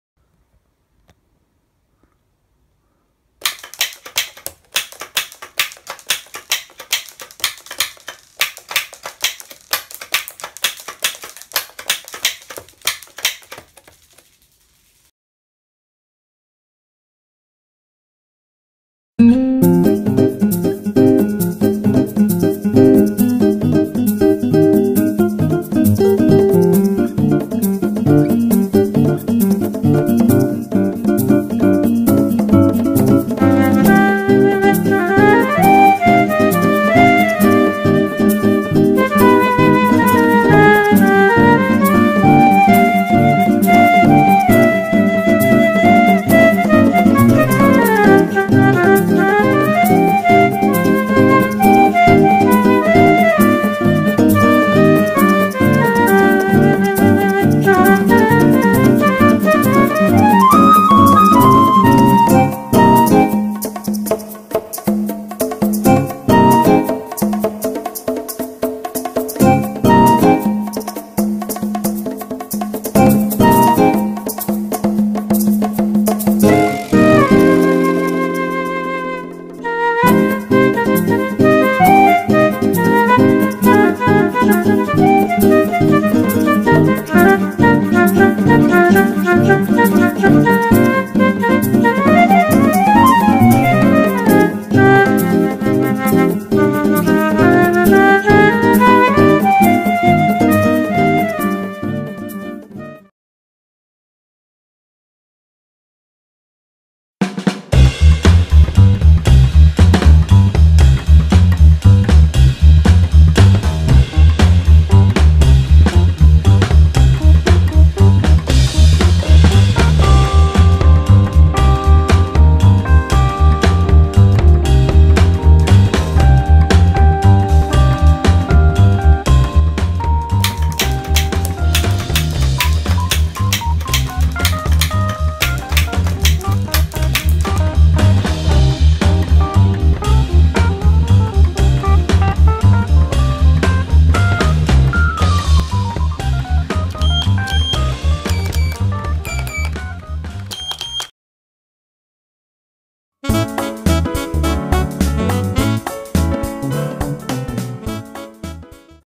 【キーボード改造】文字を打つキーボードを作ろう！
キーボード操作でBB弾を飛ばす装置を作りました。
弾を打つ以外にも、色々おまけ機能を付けております。

BGMはMusMus様よりお借りしました。